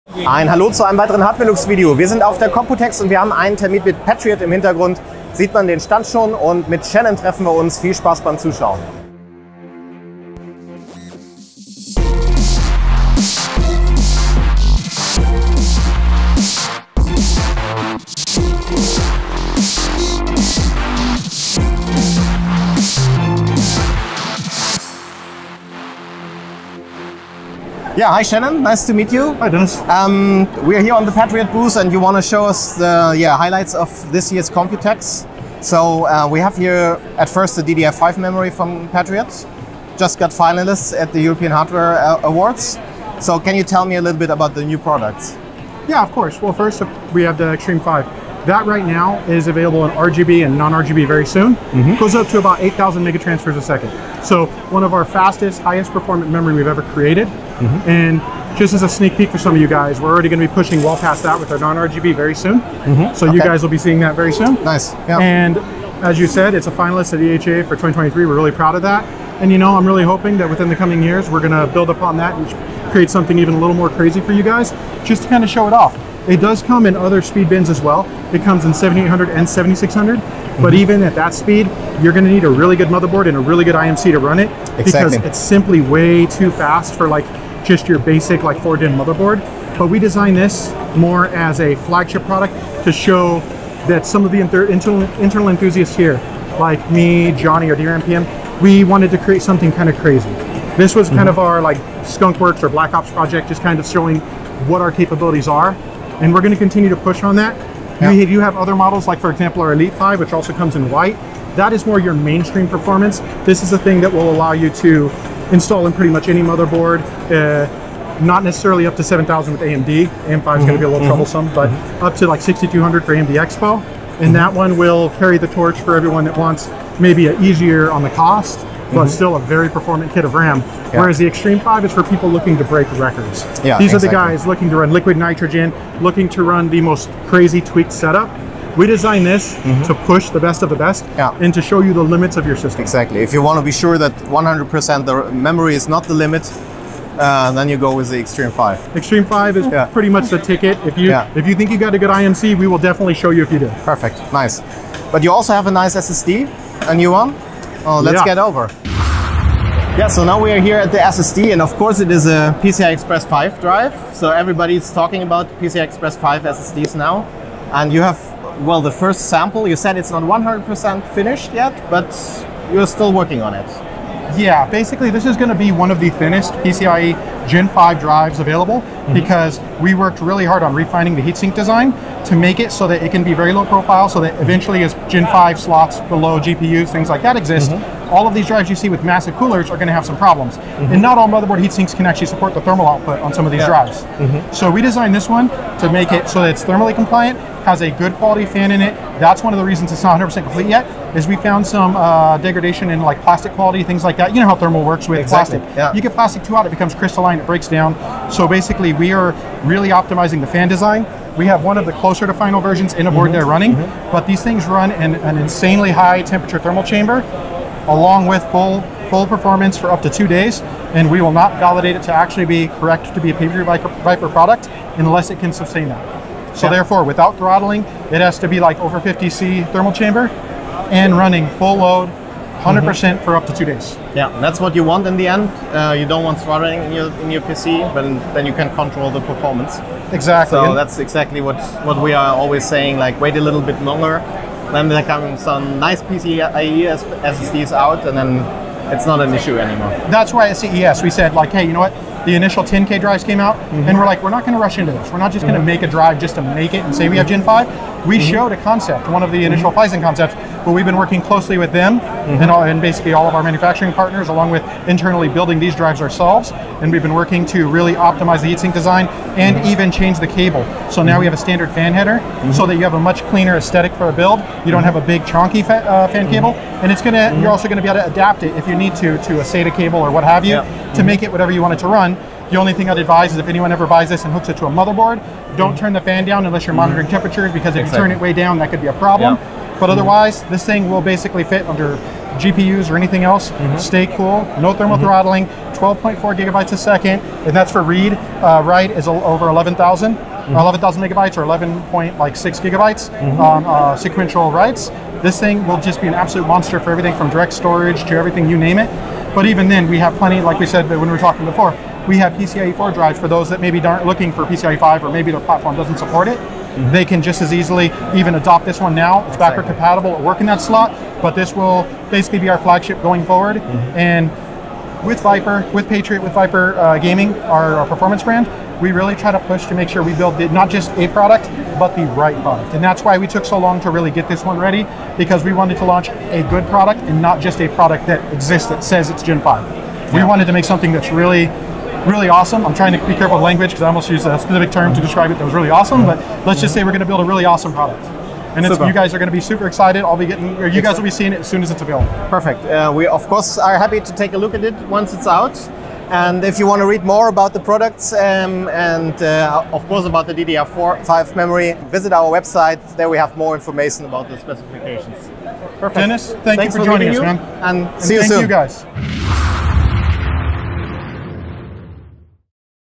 Patriot auf der Computex 2023: Viper-Xtreme-5-RAM und PV553-SSD
Bei Patriot konnten wir uns auf dem Stand DDR5-RAM aus der Viper Xtreme 5-Serie anschauen. Außerdem gab es die Viper PV553-SSD zusehen, die für eine PCIe5-SSD besonders kompakt ausfallen soll.

Alle Infos in unserem Video.